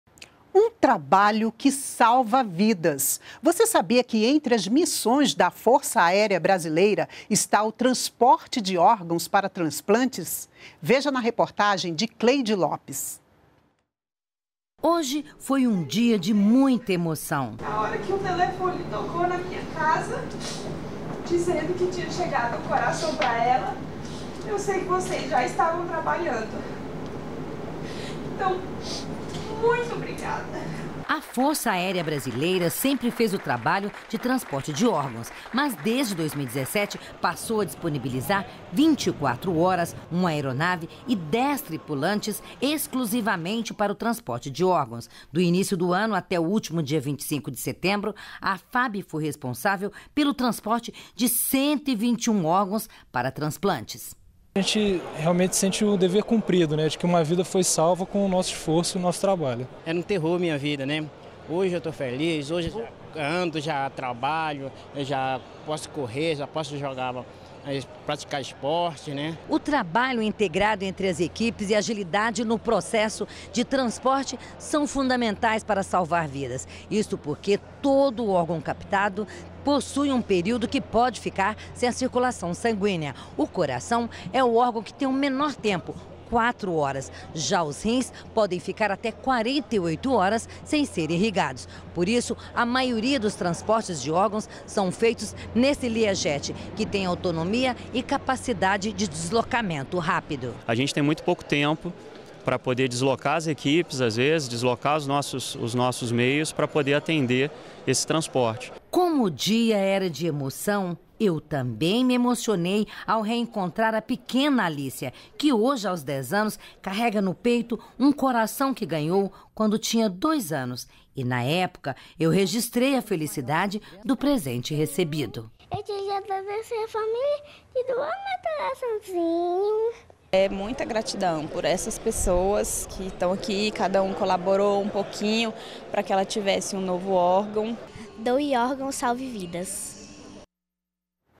Força Aérea se dedica ao transporte de órgãos para transplante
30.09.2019 - Um trabalho que  salva vidas.  Você sabia que, entre as missões da Força Aérea Brasileira, está o transporte de órgãos, para transplante ? Veja na reportagem de Cleide Lopes